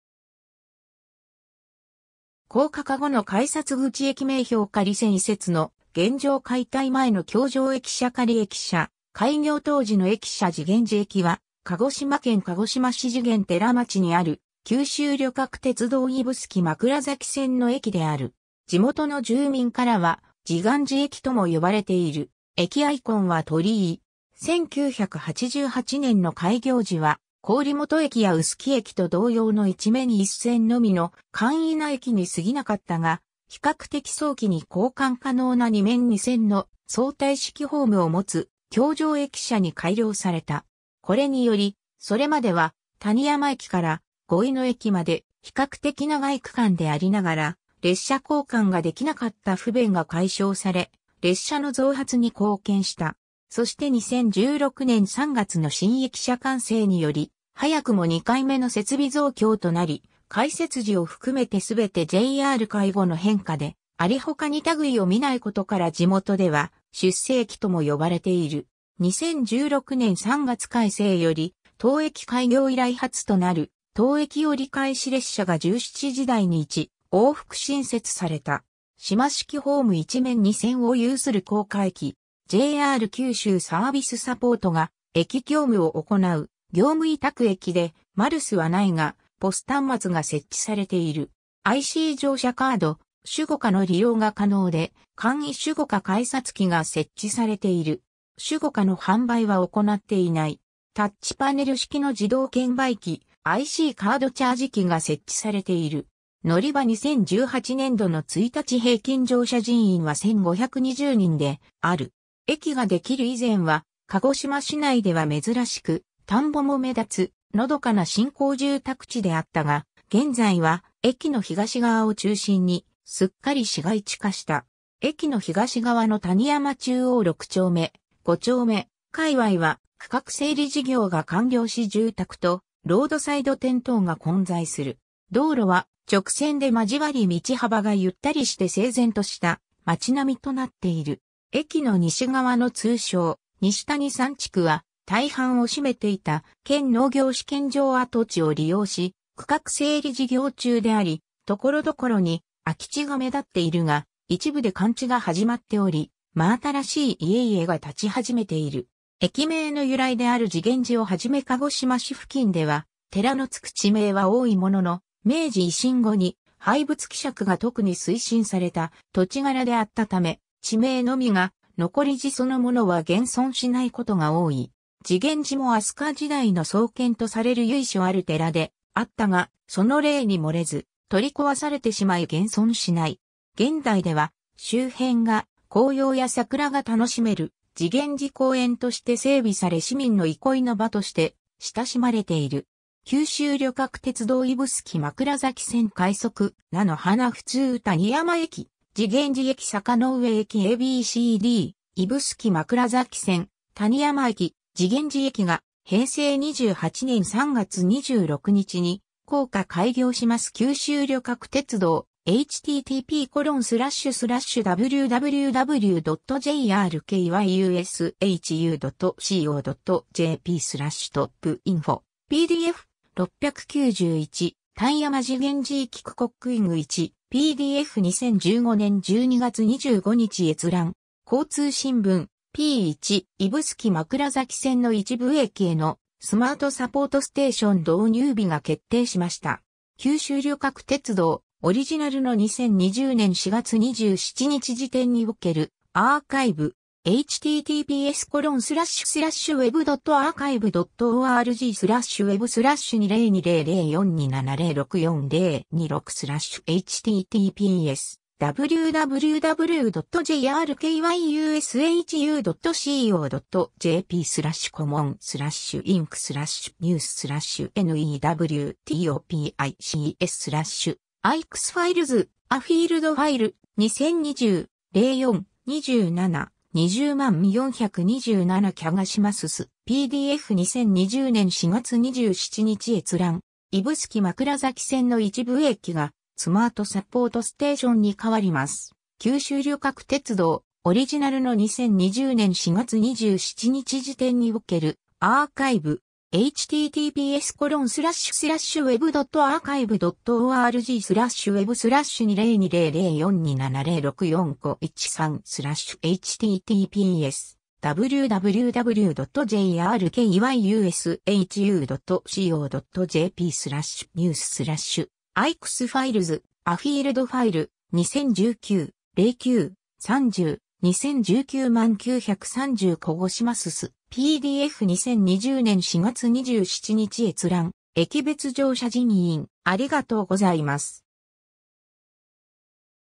慈眼寺駅
高架化後の改札口（2016年3月29日） 駅名標（2007年8月13日） 仮線移設の現状（2010年3月13日） 解体前の橋上駅舎（2005年7月） 仮駅舎（2013年8月） 開業当時の駅舎（1988年3月） 慈眼寺駅（じげんじえき）は、鹿児島県鹿児島市慈眼寺町にある、九州旅客鉄道（JR九州）指宿枕崎線の駅である。
地元の住民からは「じがんじ駅」とも呼ばれている。
駅アイコンは鳥居。
 1988年の開業時は、郡元駅や宇宿駅と同様の1面1線のみの簡易な駅に過ぎなかったが、比較的早期に交換可能な2面2線の相対式ホームを持つ橋上駅舎に改良された。
これにより、それまでは谷山駅から五位野駅まで比較的長い区間でありながら列車交換が出来なかった不便が解消され、列車の増発に貢献した。
そして2016年3月の新駅舎完成により早くも2回目の設備増強となり、開設時を含めて全てJR化以後の変化であり他に類を見ないことから地元では出世駅とも呼ばれている。
2016年3月改正より当駅開業以来初となる当駅折り返し列車が17時台に1往復新設された。
 島式ホーム1面2線を有する高架駅。
 JR九州サービスサポートが駅業務を行う業務委託駅で、マルスは無いがPOS端末が設置されている。
 IC乗車カード「SUGOCA」の利用が可能で（相互利用可能ICカードはSUGOCAの項を参照）、簡易SUGOCA改札機が設置されている。
SUGOCAの販売は行なっていない。
 タッチパネル式の自動券売機（ICカード・オレンジカード非対応）、ICカードチャージ機が設置されている。
 のりば 2018年（平成30年）度の1日平均乗車人員は1,520人である。
 駅が出来る以前は、鹿児島市内では珍しく田圃も目立つのどかな新興住宅地であったが、現在は、駅の東側を中心にすっかり市街地化した。
 駅の東側の谷山中央6丁目、5丁目界隈は、区画整理事業が完了し住宅とロードサイド店等が混在する。
道路は直線で交わり道幅がゆったりして整然とした街並となっている。
駅の西側の通称「西谷山地区」は、大半を占めていた県農業試験場跡地を利用し区画整理事業中であり、所々に空き地が目立っているが、一部で換地が始まっており真新しい家々が建ち始めている。
 駅名の由来である慈眼寺をはじめ鹿児島市付近では「寺」の付く地名は多いものの、明治維新後に廃仏毀釈が特に推進された土地柄であったため、地名のみが残り寺そのものは現存しないことが多い。
慈眼寺も飛鳥時代の創建とされる由緒ある寺であったが、その例に漏れず取り壊されてしまい現存しない。
現代では周辺が紅葉や桜が楽しめる慈眼寺公園として整備され市民の憩いの場として親しまれている。
九州旅客鉄道 指宿枕崎線 ■快速「なのはな」・■普通 谷山駅 - 慈眼寺駅 - 坂之上駅 ^ a b c d “指宿枕崎線 谷山駅・慈眼寺駅が平成28年3月26日に高架開業します” (PDF) (プレスリリース), 九州旅客鉄道, (2015年12月24日), 2015年12月25日閲覧。
 ^ 交通新聞 (交通新聞社): p. 1. (2012年12月4日) ^ “指宿枕崎線の一部駅への「スマートサポートステーション」導入日が決定しました。
” (日本語) (PDF) (プレスリリース), 九州旅客鉄道, (2020年4月27日), オリジナルの2020年4月27日時点におけるアーカイブ。
2020年4月27日閲覧。
 ^ “指宿枕崎線の一部駅が「スマートサポートステーション」に変わります” (日本語) (PDF) (プレスリリース), 九州旅客鉄道, (2019年9月30日), オリジナルの2020年4月27日時点におけるアーカイブ。